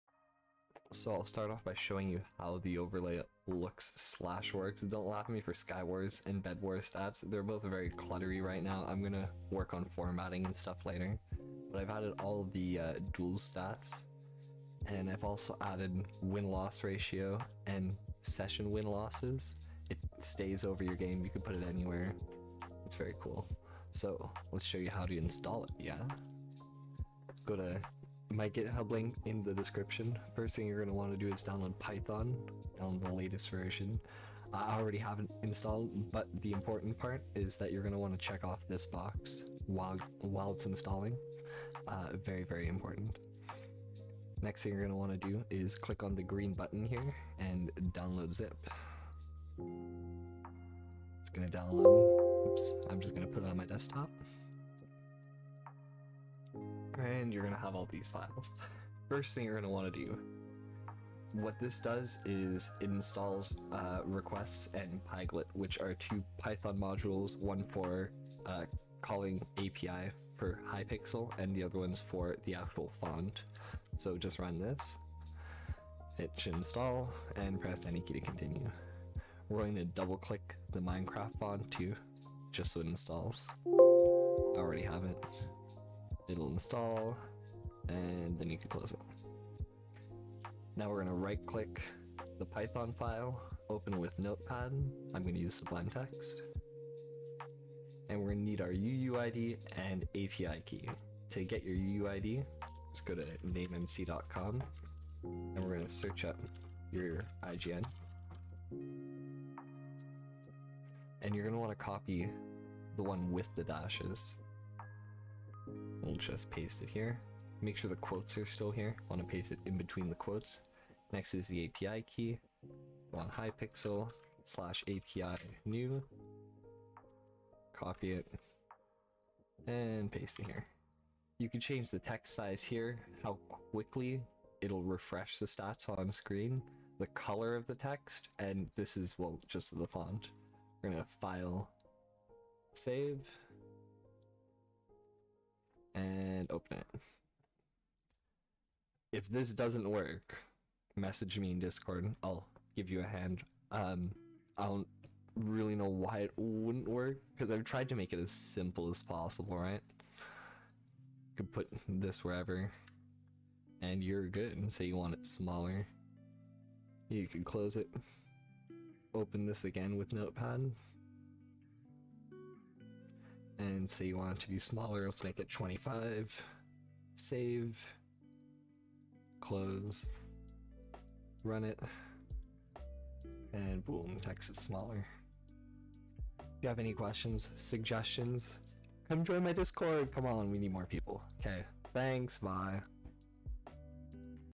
[RELEASE] Hypixel Stats Overlay - Works on ANY Client (+SESSION WINS!!!)
0:00 - Showcase
0:26 - Installation
2:44 - Resize Text